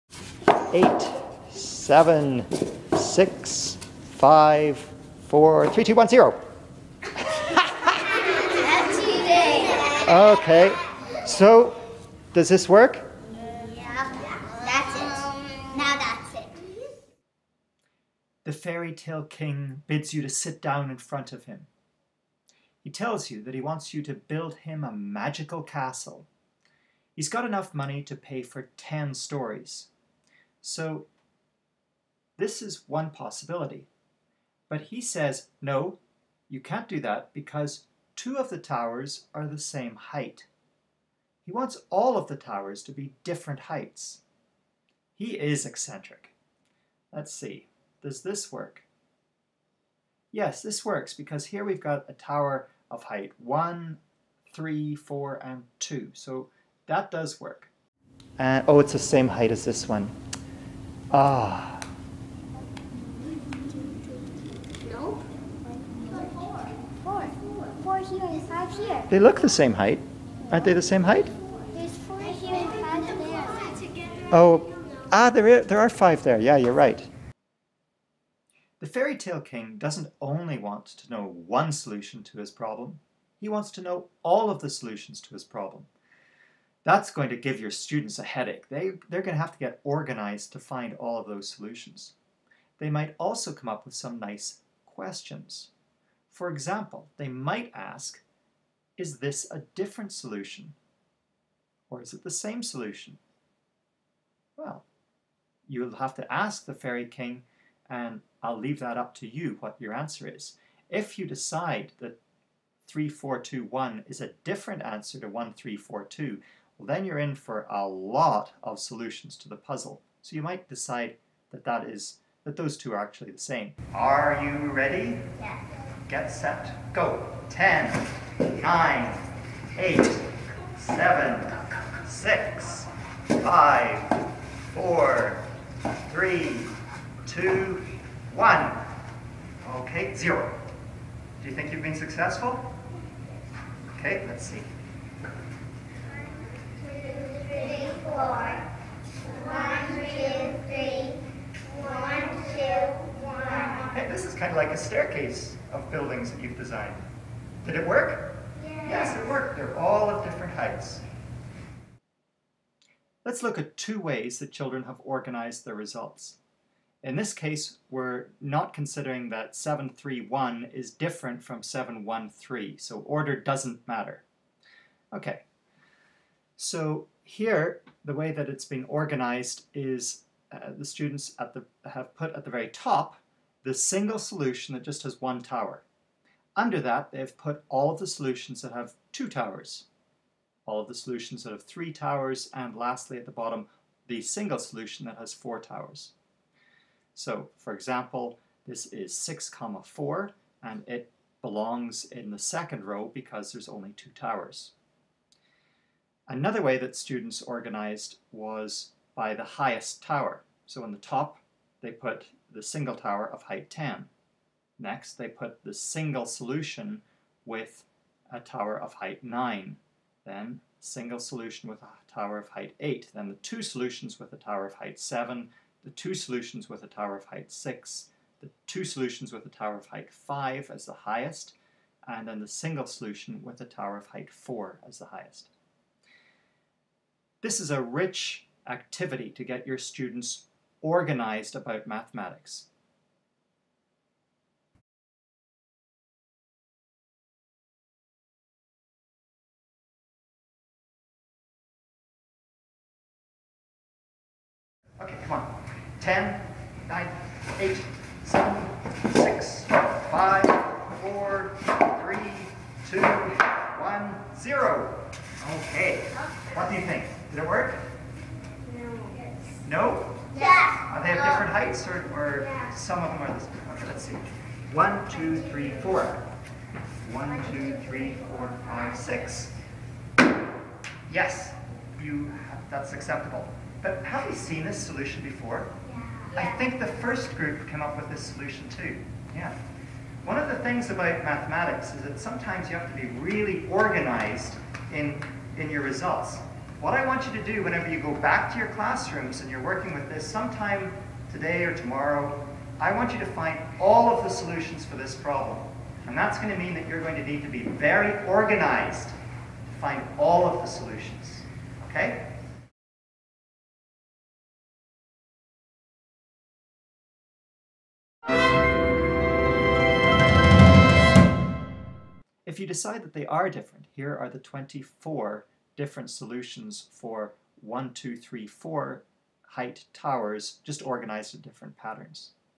Towers of Different Height
The number of blocks is 10.  Stack them up to make any number of towers... the only rule is that the heights of all the towers must be different.  The king wants to see all the possible solutions!

Mathematics is sometimes synonymous with getting organized.  Here we get students to categorize their results of a puzzle so that they can be sure that they've found all the solutions.